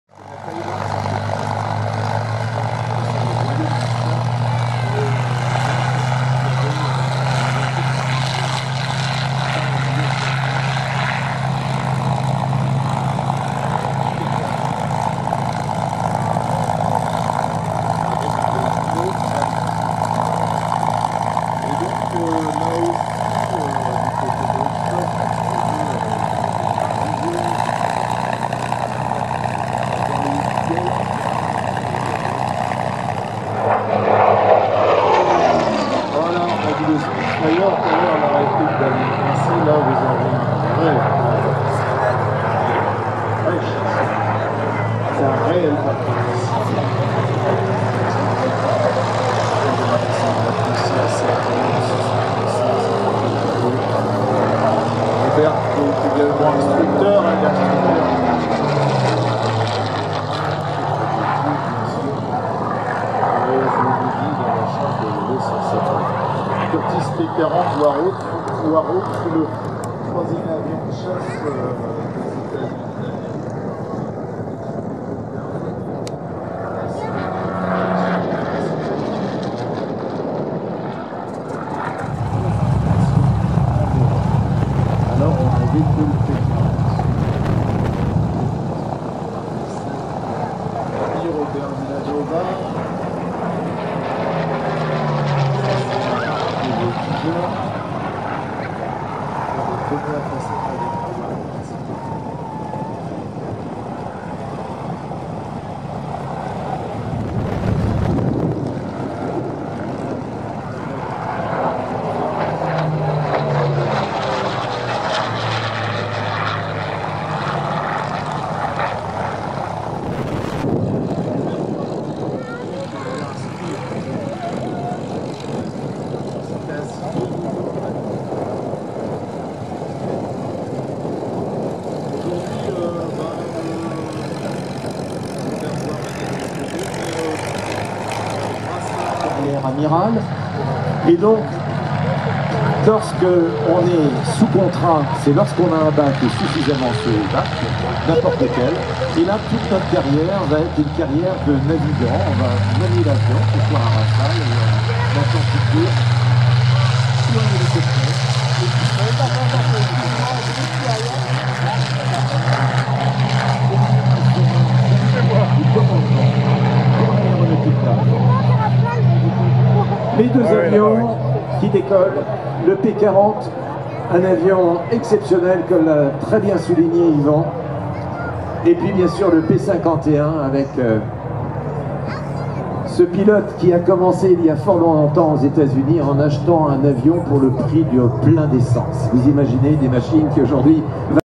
P40 - Fou d'ailes - Cholet Le Pontreau 2016
Démonstration en vol du Curtiss P-40 Warhawk durant la fête aérienne "Fou d'ailes" 2016 à l'aérodrome de Cholet Le Pontreau.